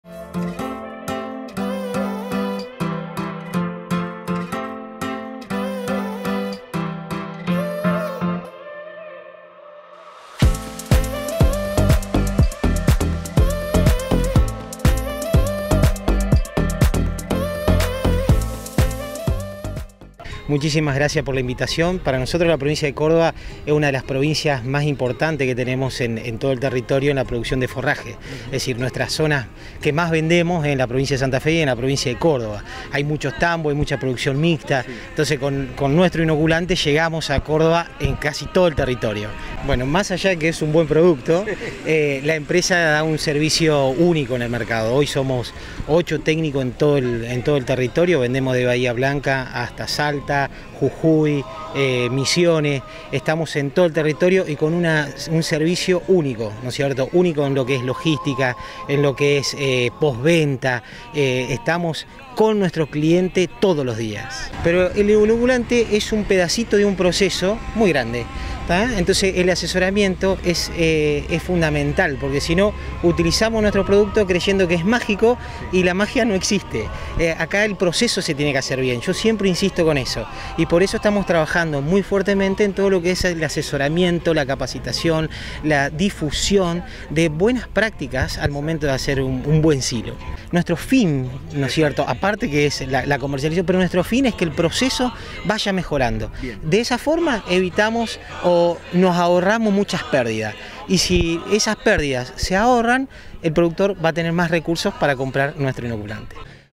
Bemix presente en la Dinámica de Expo Villa 2021
Se realizó en Villa del Rosario, la Dinámica Expo Villa 2021. Se trata de la primera Jornada a Campo que se realiza en nuestra provincia, luego de la pandemia por el Covid.